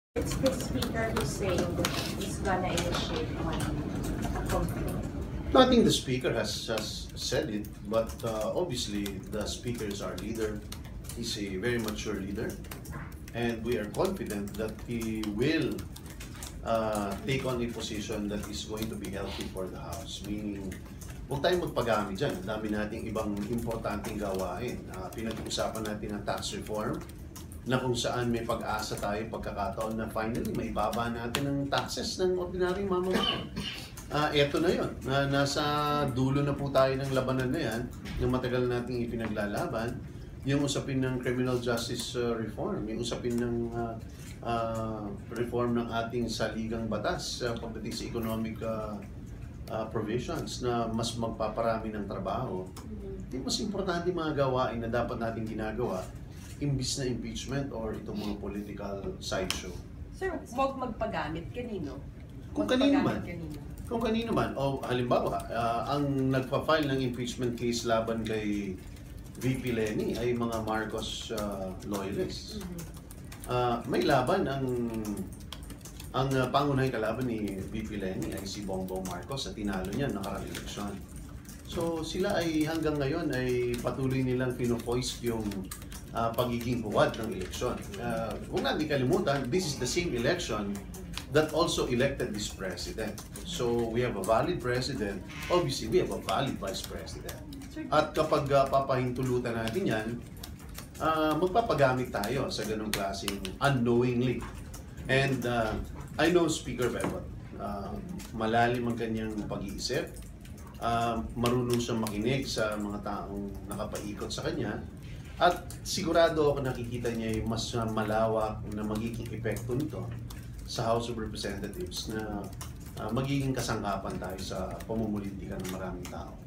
Quimbo confident Alvarez will be prudent on impeachment complaint
More videos on Rappler Follow Rappler for the latest news in the Philippines and across the globe. Liberal Party (LP) stalwart and Deputy Speaker Miro Quimbo believes that Speaker Pantaleon Alvarez will consider the greater interest of the House in deciding whether or not to file an impeachment complaint against Vice President Leni Robredo. Full story